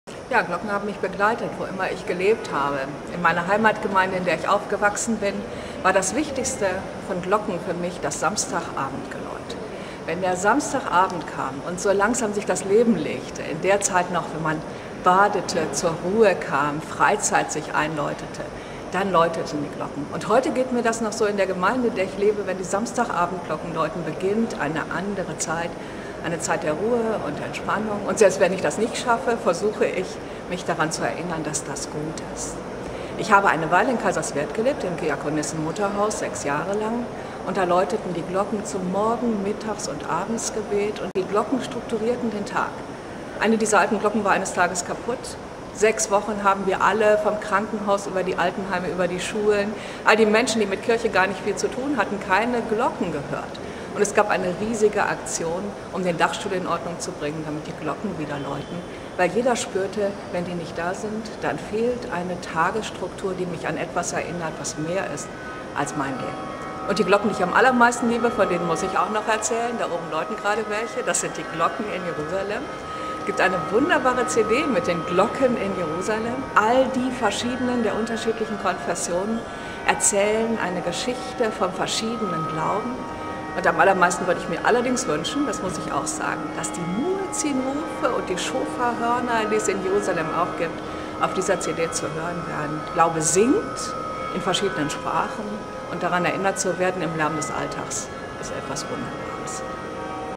Cornelia Coenen-Marx: Was Glocken mir bedeuten.
Über Jerusalem, die Kaiserswerther Diakonie und das Einläuten des Sonntags spricht Oberkirchenrätin Cornelia Coenen-Marx in diesem Film. Sie ist seit 2007
Referentin Sozial- und Gesellschaftspolitik sowie Verbindung Kirche und Diakonie im Kirchenamt der EKD in Hannover. Zuvor war die Theologin u.a. Theologischer Vorstand der Kaiserswerther Diakonie und Vorsteherin der Kaiserswerther Schwesternschaft.